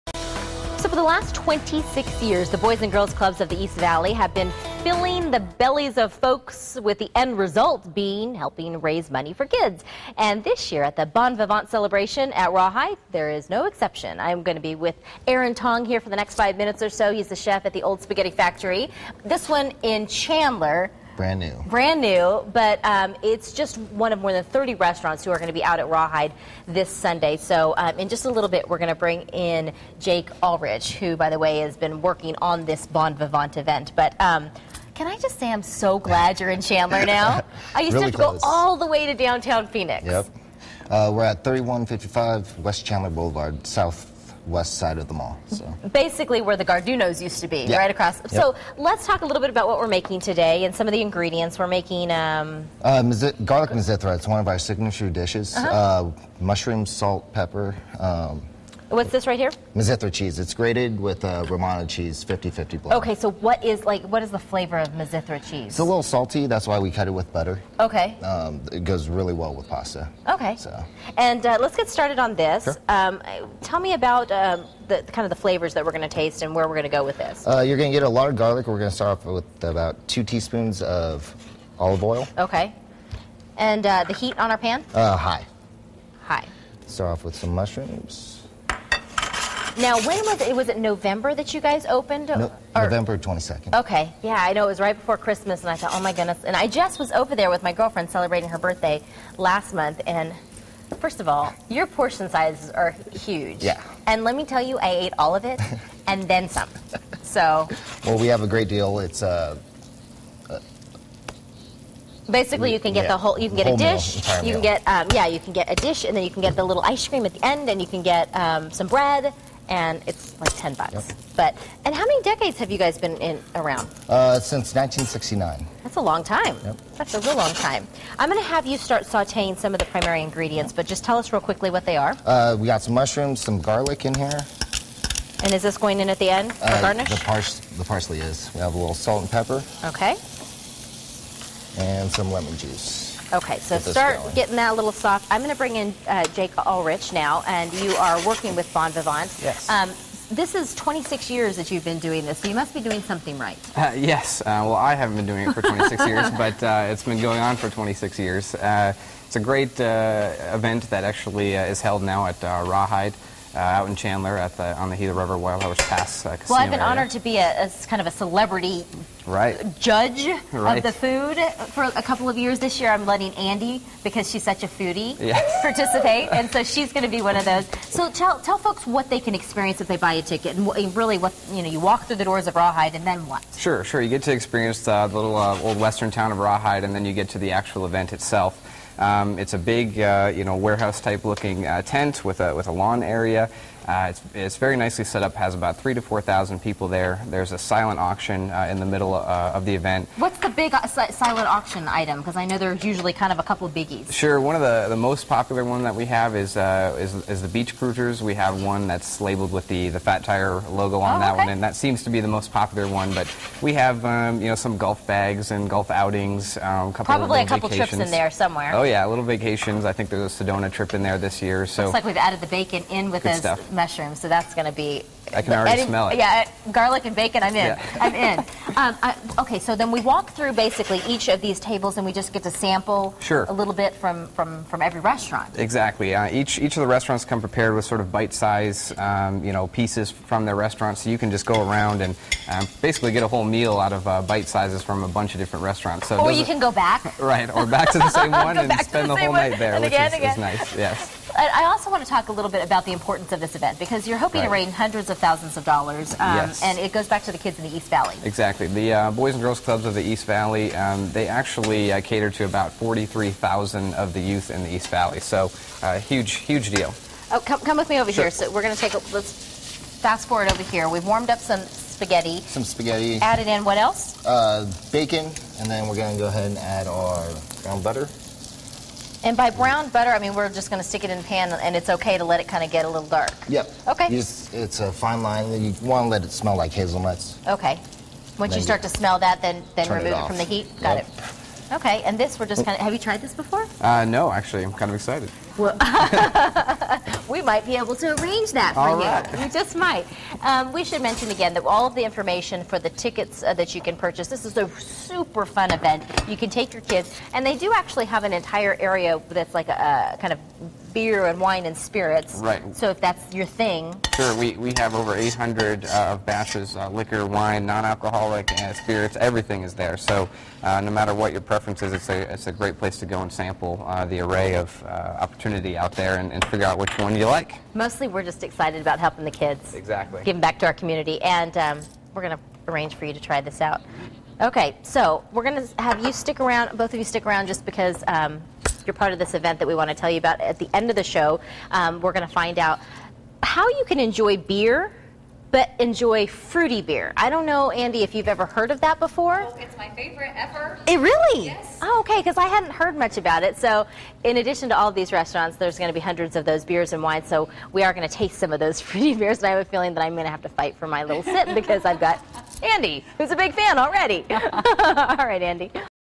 Signature recipe from Old Spaghetti Factory
A delicious Garlic Mizithra recipe is one many at the 26th annual Bon Vivant.